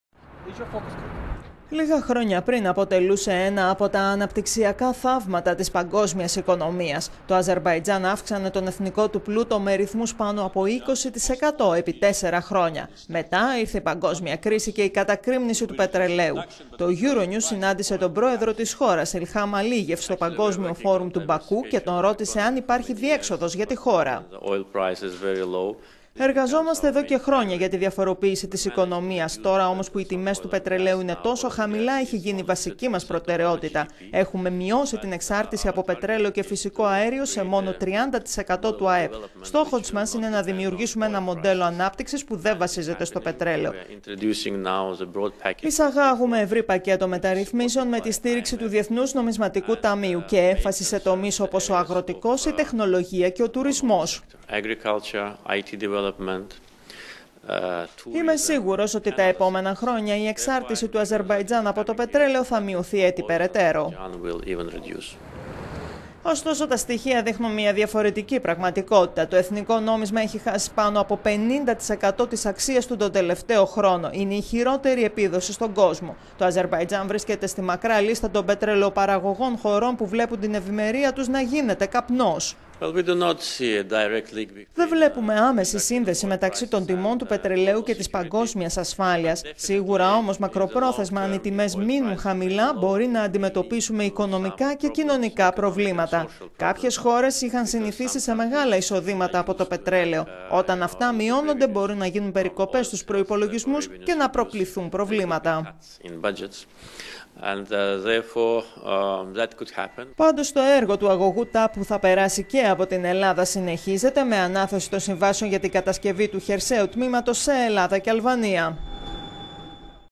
Αζερμπαϊτζάν: το πετρέλαιο, η κρίση, οι υποσχέσεις - economy
Μέχρι λίγα χρόνια πριν, αποτελούσε ένα από τα «αναπτυξιακά θαύματα» της παγκόσμιας οικονομίας.
Το Αζερμπαϊτζάν αύξανε τον εθνικό του πλούτο με ρυθμούς πάνω από 20%, επί τέσσερα χρόνια.
Μετά ήρθε η παγκόσμια κρίση και η κατακρήμνιση του πετρελαίου.

Το euronews συνάντησε τον Πρόεδρο της χώρας, Ιλχάμ Αλίγιεφ, στο Παγκόσμιο Φόρουμ του Μπακού (που διοργάνωσε το Διεθνές Κέντρο Nizami Ganjavi) και τον ρώτησε αν υπάρχει διέξοδος για την χώρα:

«Εργαζόμαστε, εδώ και χρόνια, για τη διαφοροποίηση της οικο…